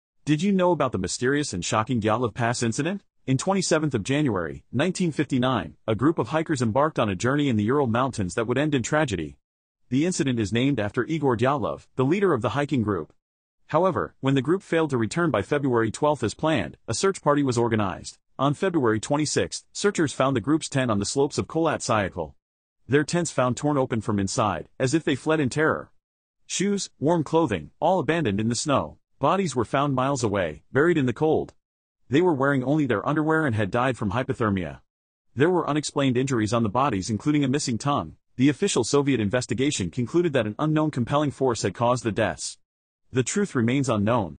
Something Strange Happened in These Mountains
In 1959, a group of hikers embarked on a journey into the Ural Mountains, seeking adventure and exploration. Little did they know, their trip would take a sinister turn, leaving behind an enduring mystery that still captivates us today. 🕵️‍♂️

Their campsite was discovered in disarray, the tent torn open from the inside. Something had clearly terrified them, prompting their hasty escape. ❄️

They left behind their shoes, warm clothing, and personal belongings, as if compelled to flee without a moment's hesitation. 🥾❄️

A search party was organized, desperately scouring the treacherous terrain for any sign of the missing hikers. Their bodies were eventually found, scattered miles away from the campsite, buried beneath the snow. 😔⛄️

Their deaths revealed puzzling injuries, including fractured skulls and broken ribs. The cause remains unknown, adding to the enigma that surrounds this haunting tale. 🤔

Decades have passed, yet the truth of what happened that fateful night still eludes us. The Dyatlov Pass Incident remains an enduring mystery, an eerie reminder of the unknown forces that can lurk in the wilderness. 🌬️🏔️

Image Credits:
Teodora Hadijyska/ Dyatlov Pass Website